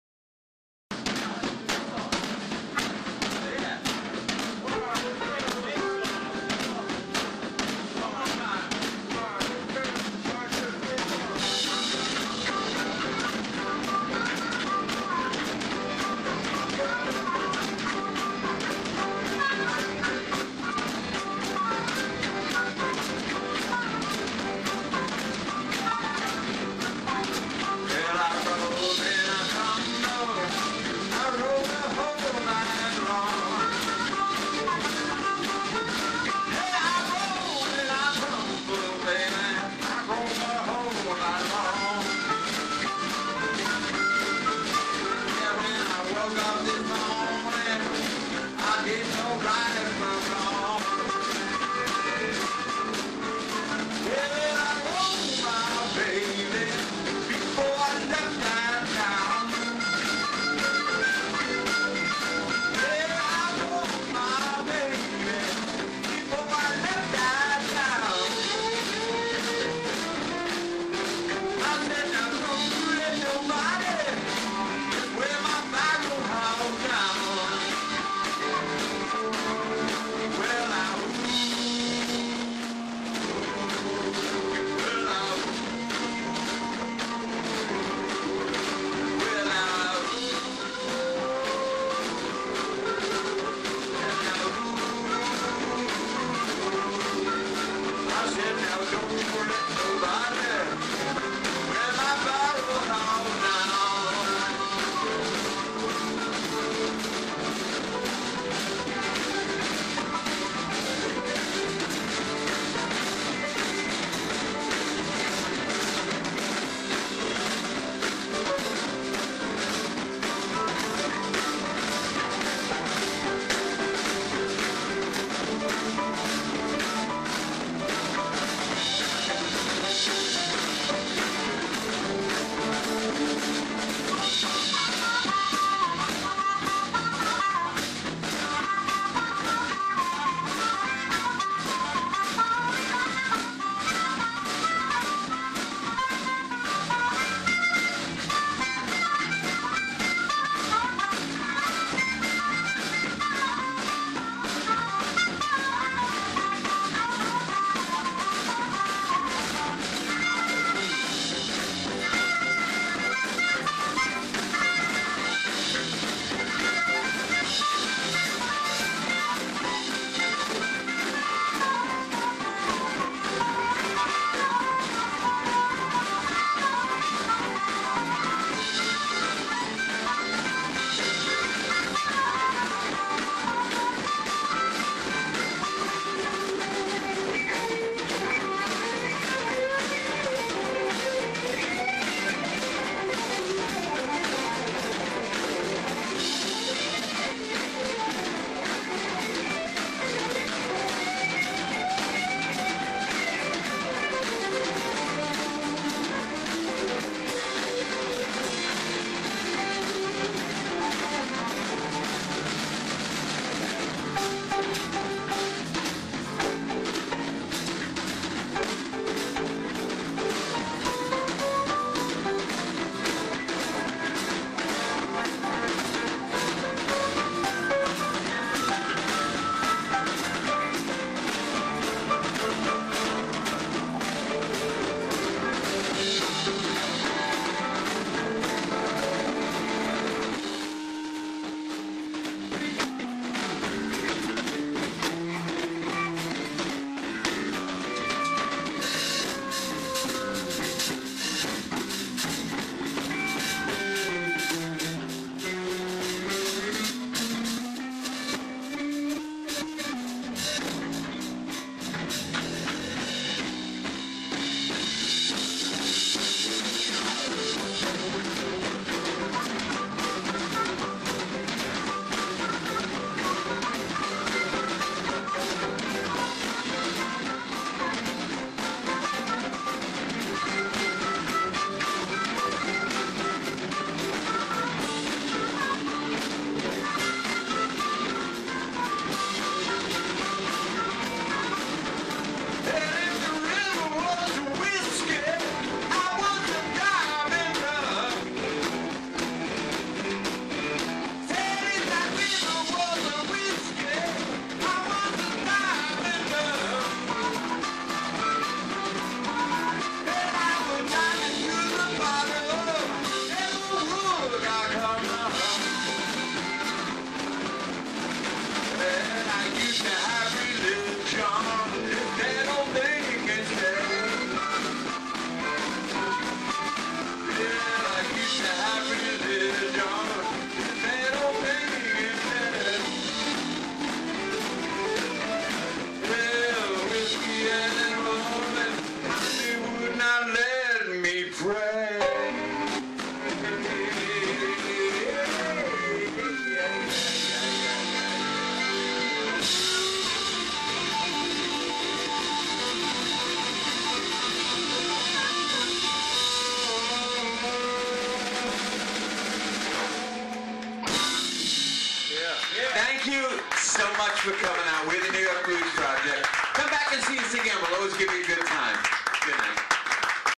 New York Blues Projeck
Jon Montgomery: Vocals
Steve Bondy: Guitar
Clint Bahr: Bass
Toby Kasovan: Piano
David Donen: Drums

recorded at : The Underground Lounge, NYC 2013 by D. Archer
Sorry for the audio. This video was taken off of a smart phone.